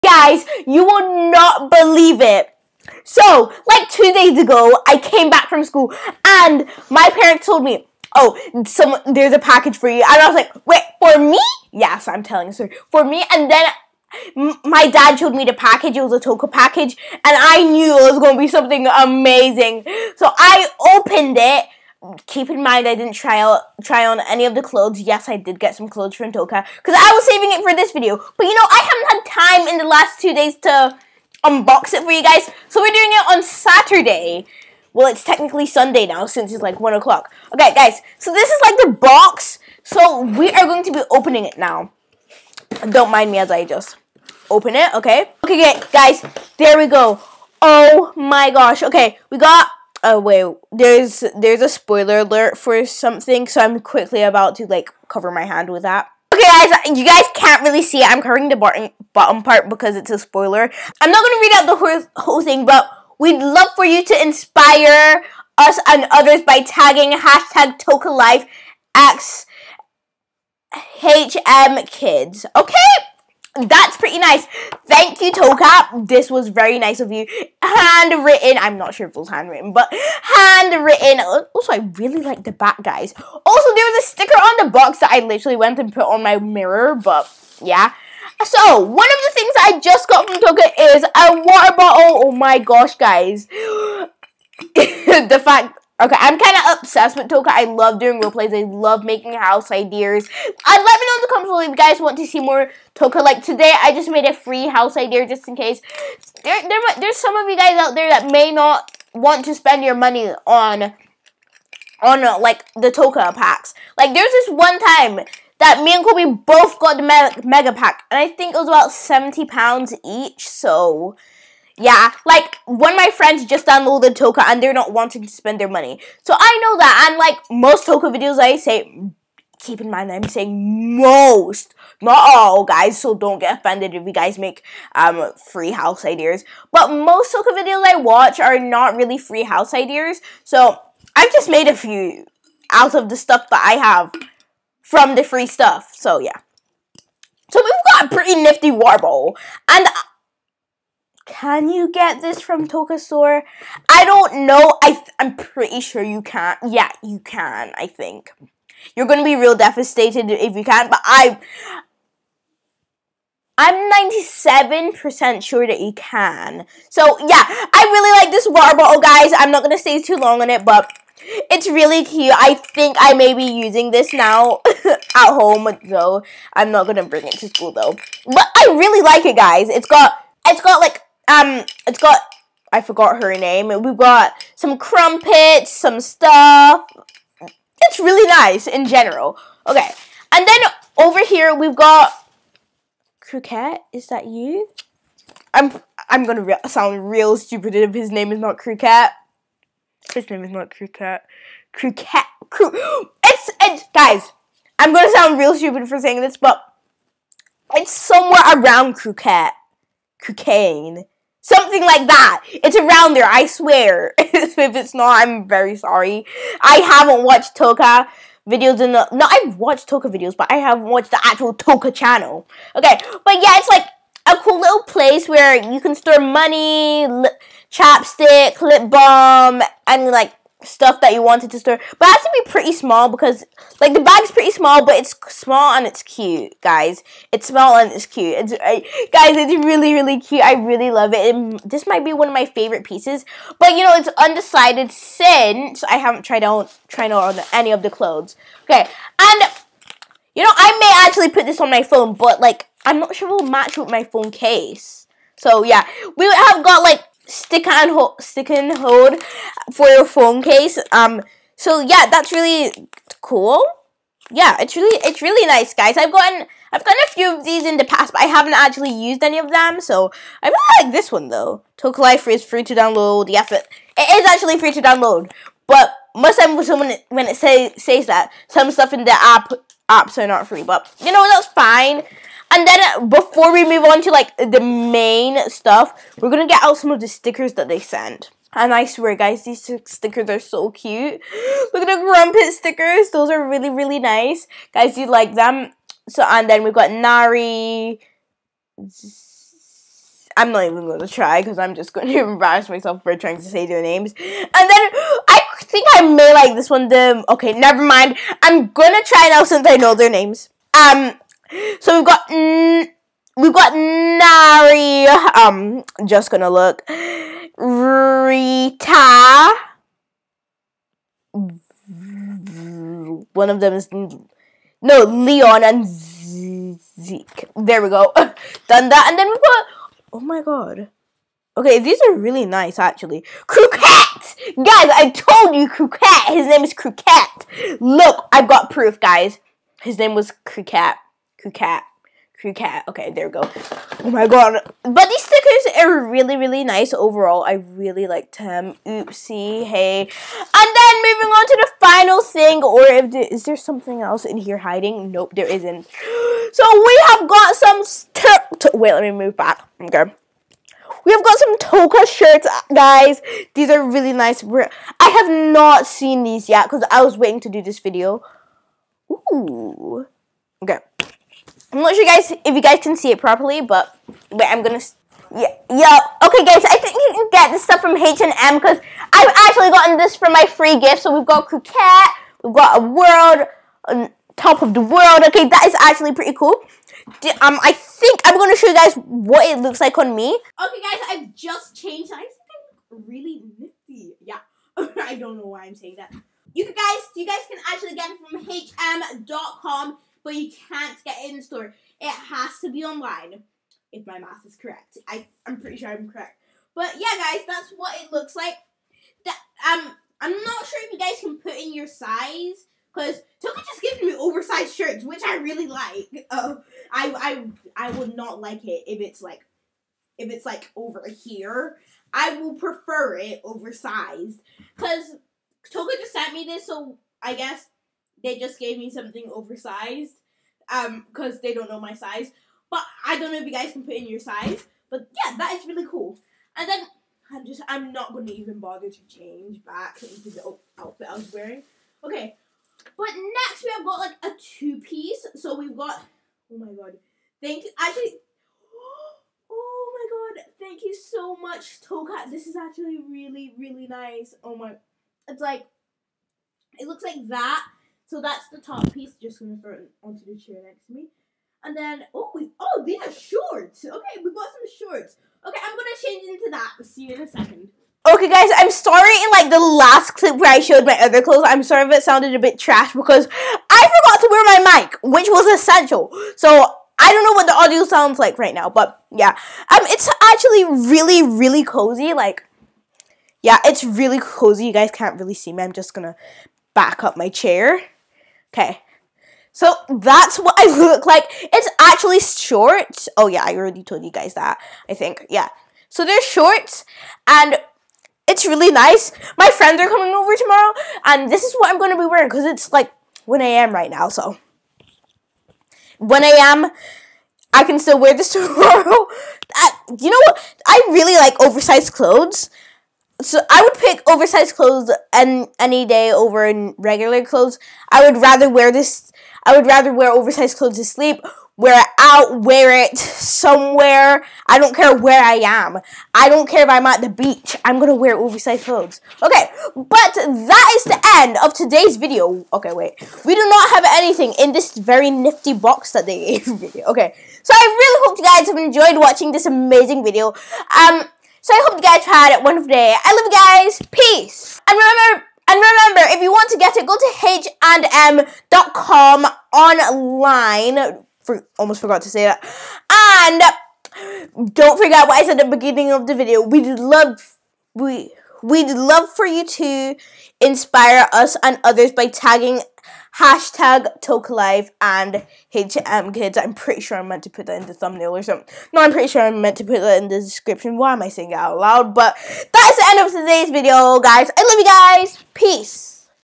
UNBOXING TOCA LIFE X H&M COLLABORATION GOODIES!!! 🥰
TOCA LIFE X H&M GOODIES

Hi My Friends, Toca Life sent me some awesome goodies. This is a collaboration between Toca life and H&M.

If you decide you want to get the collection, visit HM.COM